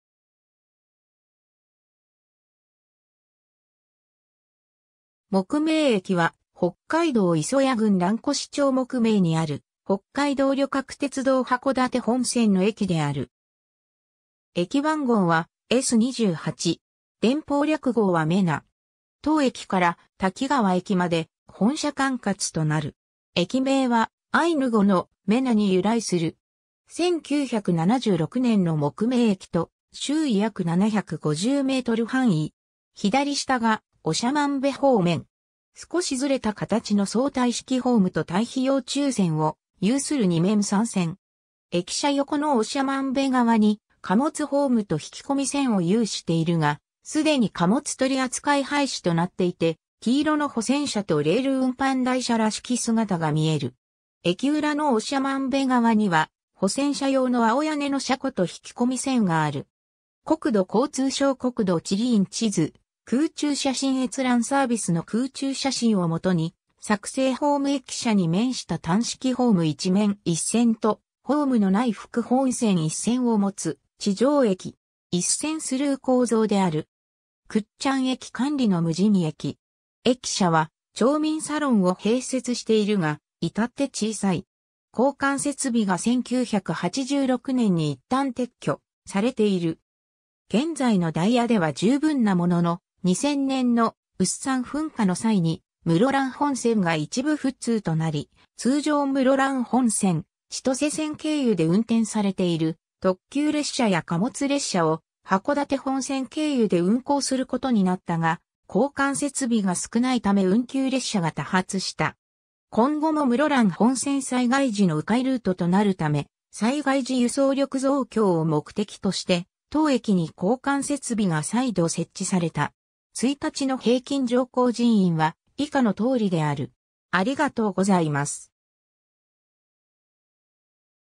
目名駅
北海道鉄道（初代）
目名駅（めなえき）は、北海道磯谷郡蘭越町目名にある北海道旅客鉄道（JR北海道）函館本線の駅である。
駅番号はS28。
電報略号はメナ。
 当駅から滝川駅(滝川市栄町4丁目9)まで本社管轄となる。
 駅名はアイヌ語の「メナ」（細流、支流）に由来する。
 1976年の目名駅と周囲約750m範囲。
左下が長万部方面。
少しずれた形の相対式ホームと待避用中線を有する2面3線。
駅舎横の長万部側に貨物ホームと引込み線を有しているが、既に貨物取扱廃止となっていて、黄色の保線車とレール運搬台車らしき姿が見える。
駅裏の長万部側には、保線車用の青屋根の車庫と引込み線がある。
国土交通省 国土地理院 地図・空中写真閲覧サービスの空中写真を基に作成 ホーム（2017年9月） 駅舎に面した単式ホーム1面1線と、ホームのない副本線1線を持つ地上駅。
一線スルー構造である。
倶知安駅管理の無人駅。
駅舎は「町民サロン」を併設しているが、いたって小さい。
 交換設備が1986年に一旦撤去されている。
現在のダイヤでは充分なものの、2000年の有珠山噴火の際に室蘭本線が一部不通となり、通常室蘭本線・千歳線経由で運転されている特急列車や貨物列車を函館本線（長万部駅 - 札幌駅間）経由で運行することになったが、交換設備が少ないため運休列車が多発した。
今後も室蘭本線災害時の迂回ルートとなるため、災害時輸送力増強を目的として当駅に交換設備が再度設置された。
 1日の平均乗降人員は以下の通りである。